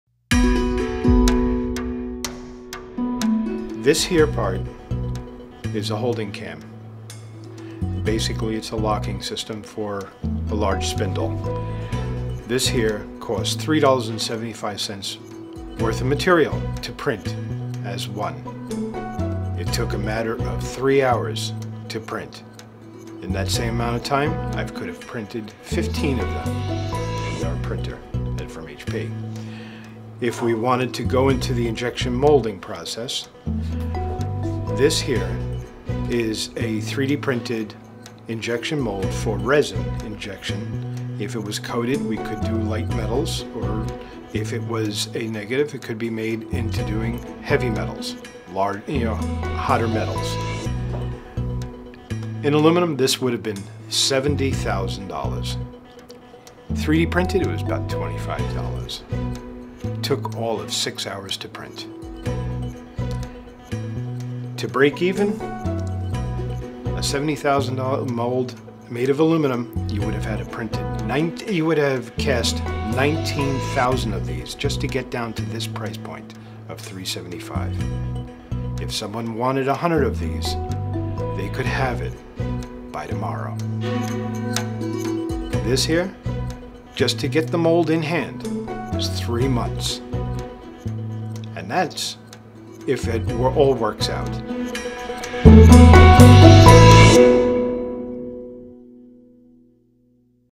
HP 3D Print: On-demand production that's better, faster, at less cost
Future Tech shows how HP's Multi Jet Fusion 3D printer can slash the costs and time for production.